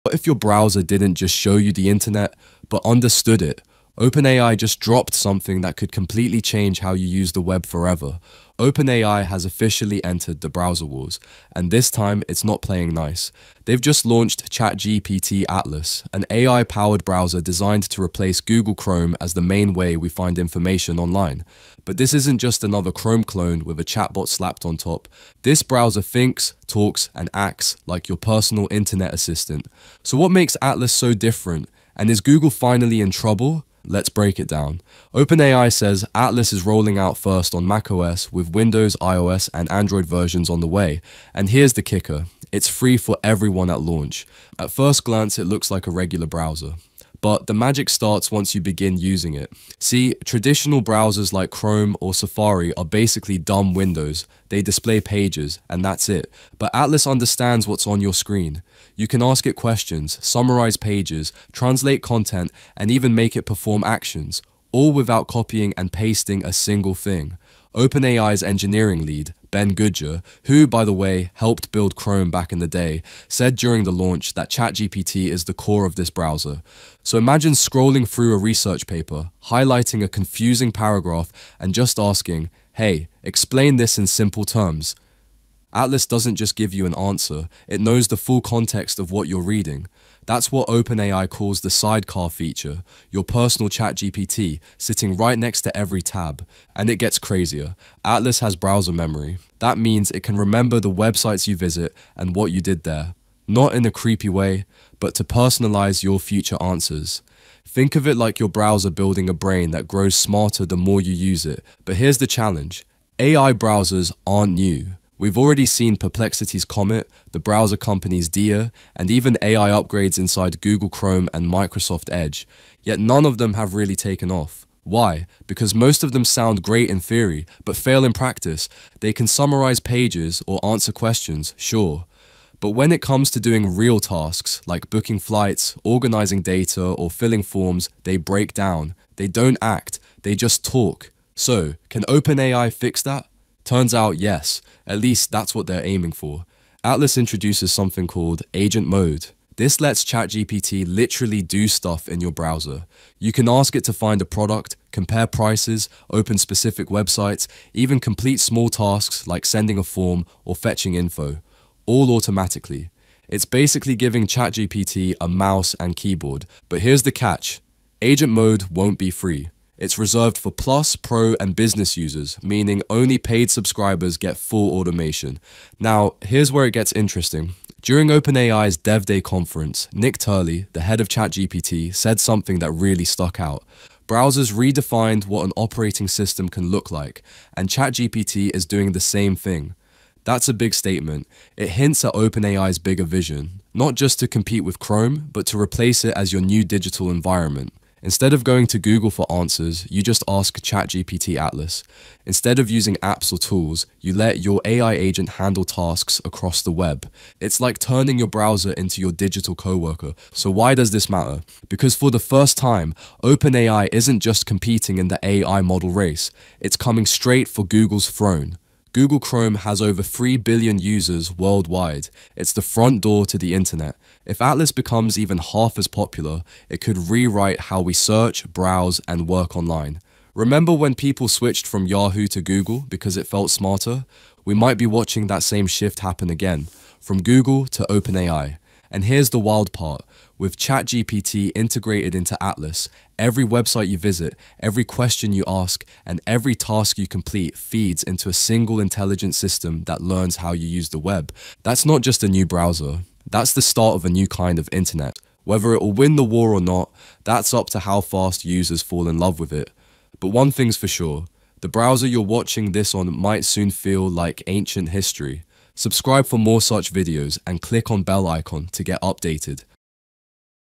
ChatGPT Atlas Explained: OpenAI’s New AI-Powered Browser vs Google Chrome
OpenAI just launched its first AI-powered web browser — ChatGPT Atlas, and it could completely change how we use the internet.

In this video, we break down everything you need to know about ChatGPT Atlas — how it works, what makes it different from Chrome, and why it might be the biggest threat Google has ever faced.

We’ll cover:
- What ChatGPT Atlas is and how it integrates AI into your browsing experience
- The sidecar feature that lets ChatGPT understand every page you visit
- The agent mode that can actually perform tasks for you online
- Why OpenAI is turning your browser into your personal digital assistant

By the end of this video, you’ll know exactly how AI-powered browsers are reshaping the future of the web — and what this means for Chrome, Edge, and Perplexity’s Comet.

💡 If you’re into AI tools, OpenAI news, or next-gen tech innovations, this one’s a must-watch.